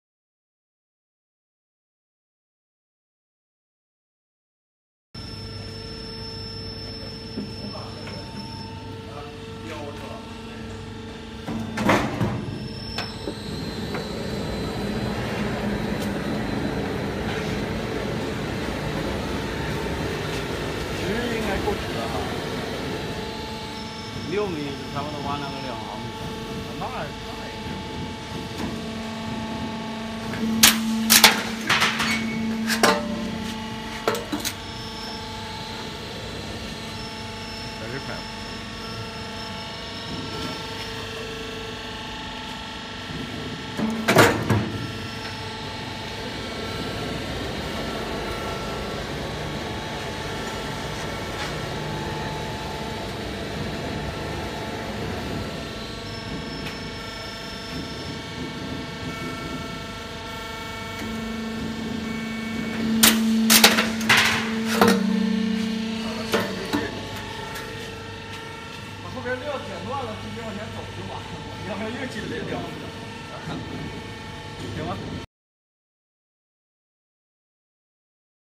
[STEELMAMA]Hat purlin roll forming machine
One machine for 3 sizes hat purlin by changing rollers